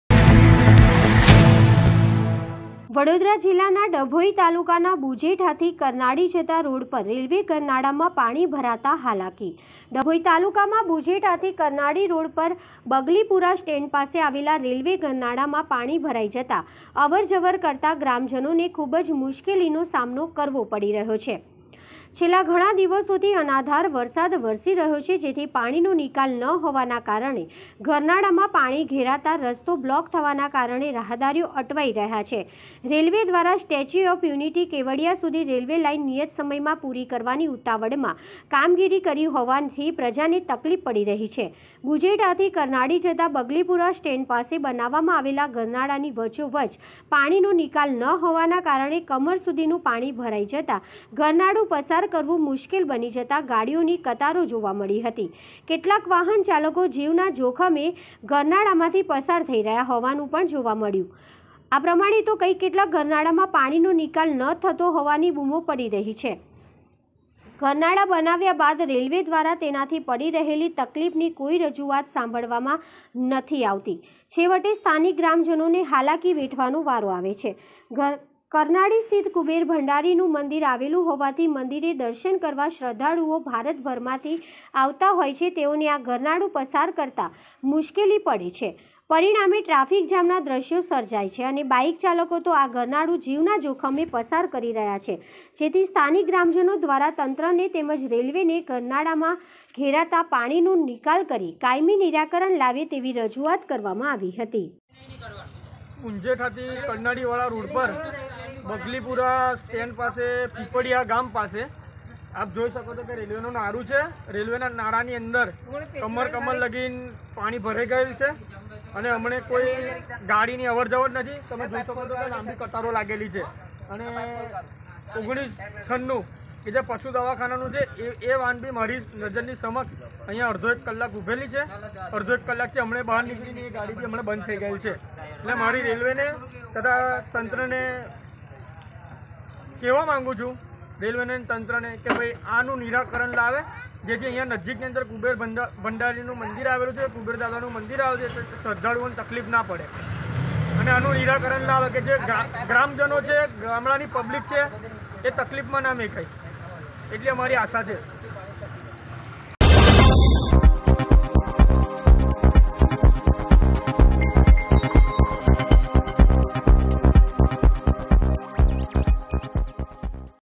વડોદરા જિલ્લાના ડભોઇ તાલુકા બુજેઠા થી કરનાળી જતા રોડ પર રેલવે ગરનાળા માં પાણી ભરાતા હાલાકી
ડભોઇ તાલુકા માં બુજેઠા થી કરનાળી રોડ પર બગલીપુરા સ્ટેન્ડ પાસે આવેલ રેલવે ગરનાળા માં પાણી ભરાઈ જતા અવર જવર કરતા ગ્રામજનો ને ખુબ જ મુશ્કેલી નો સામનો કરવો પડી રહ્યો છે.છેલ્લા ઘણા દિવસો થી અનરાધાર વરસાદ વરસી રહ્યો છે.જેથી પાણી નો નિકાલ ન હોવાના કારણે ગરનાળા માં પાણી ઘેરાતા રસ્તો બ્લોક થવાના કારણે રાહદારીઓ અટવાયી રહ્યા છે.રેલવે દ્વારા સ્ટેચ્યુ ઓફ યુનિટી કેવડિયા સુધી રેલવે લાઇન નિયત સમય માં પુરી કરવા ની ઉતાવળ માં  કામગીરી કરી હોવાથી પ્રજા ને તકલીફ પડી રહી છે. બુજેઠા થી કરનાળી જતા બગલીપુરા સ્ટેન્ડ પાસે બનાવવા માં આવેલ ગરનાળા ની વચ્ચો વચ્ચ પાણી નો નિકાલ ન હોવાના કારણે કમર સુધી નું પાણી ભરાઈ જતા ગરનાળુ પસાર કરવું મુશ્કેલ બની જતા ગાડીઓ ની કતારો જોવા મળી હતી.કેટલાક વાહન ચાલકો જીવ ના જોખમે ગરનાળા માં થી પસાર થઈ રહ્યાં હોવાનું પણ જોવા મળ્યું હતું.આ પ્રમાણે તો કઈ કેટલાય ગરનાળા માં પાણી નો નિકાલ ન થતો હોવાની બુમો પડી રહી છે.ગરનાળા બનાવ્યા બાદ રેલવે દ્વારા તેનાથી પડી રહેલી તકલીફ ની કોઈ રજુઆત સાંભળવામાં નથી આવતી છેવટે સ્થાનિક ગ્રામજનો ને હાલાકી વેઠવાનો વારો આવે છે.કરનાળી સ્થિત કુબેર ભંડારી નું મંદિર આવેલું હોવાથી મંદિરે દર્શન કરવા શ્રધ્ધાળુઓ ભારત ભર માં થી આવતા હોય છે. તેઓને આ ગરનાળુ પસાર કરતા મુશ્કેલી પડે છે પરિણામે ટ્રાફિક જામ ના દ્રશ્યો સર્જાય છે.અને બાઇક ચાલકો તો આ ગરનાળુ  જીવ ના જોખમે પસાર કરી રહ્યા છે.જેથી સ્થાનિક ગ્રામજનો દ્વારા તંત્ર ને તેમજ રેલવે ને ગરનાળા માં ઘેરાતા પાણી નું નિકાલ કરી કાયમી નિરાકરણ લાવે તેવી રજુઆત કરવામાં આવી હતી.

બ્યુરો ચીફ:- હર્ષ પટેલ ડભોઇ વડોદરા